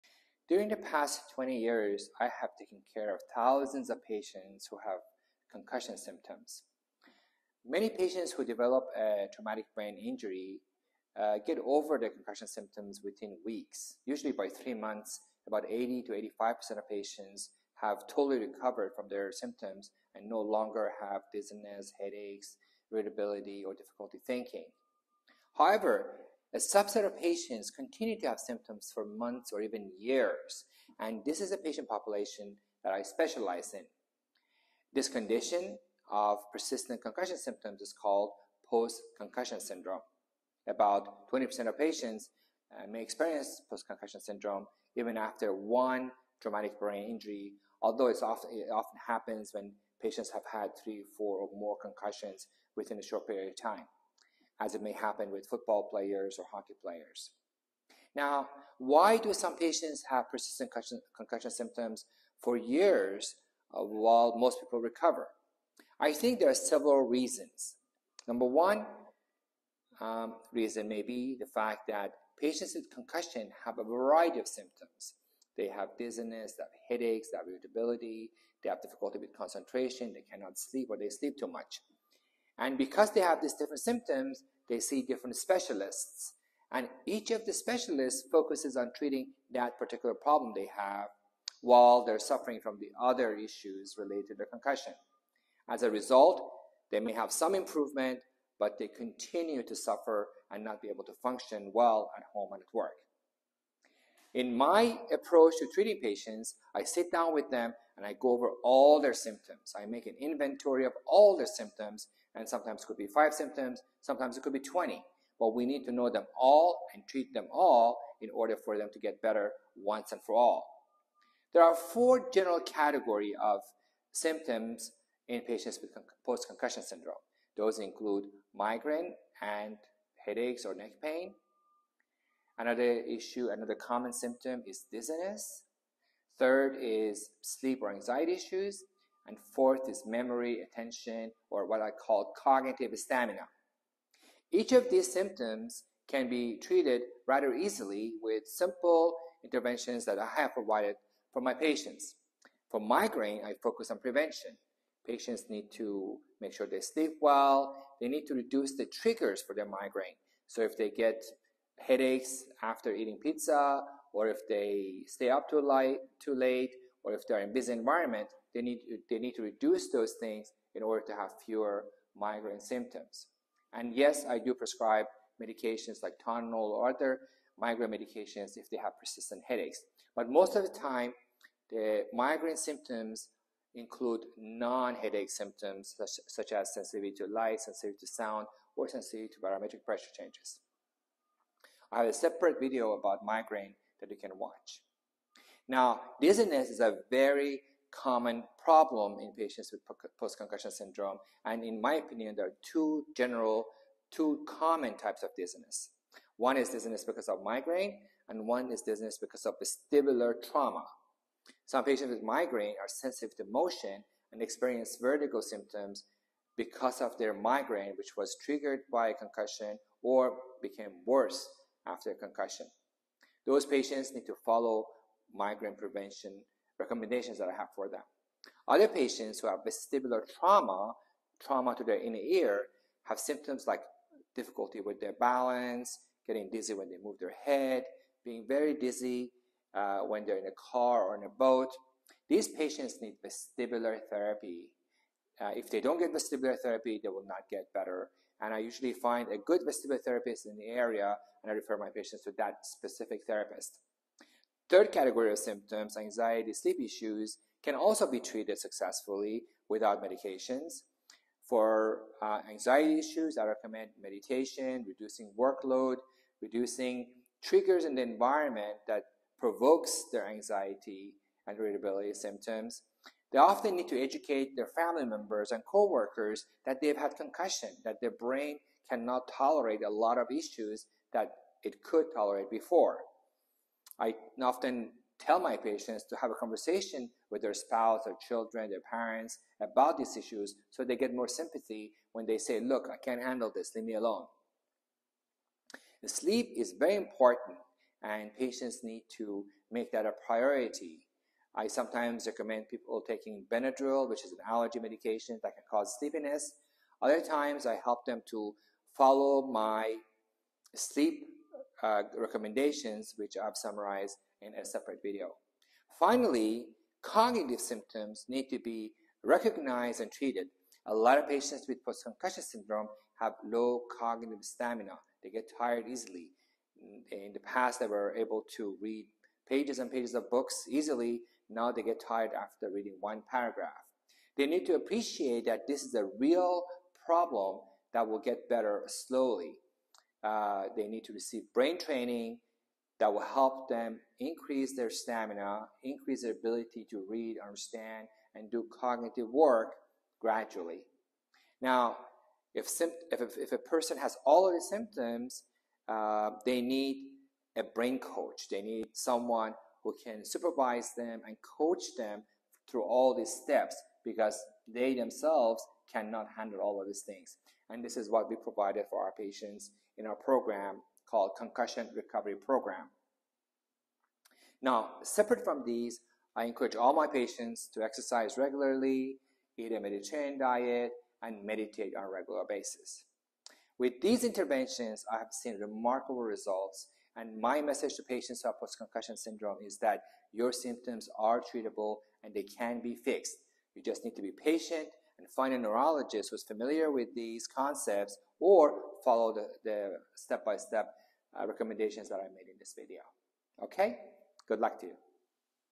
Dr. Fotuhi's "Brain Fitness Program" - Dealing with Post Concussion Syndrome
In this video, Dr. Fotuhi discusses concussions and how to treat post concussion syndrome. Persistent concussion syndromes must be treated and Dr. Fotuhi outlines the proper methods to do so.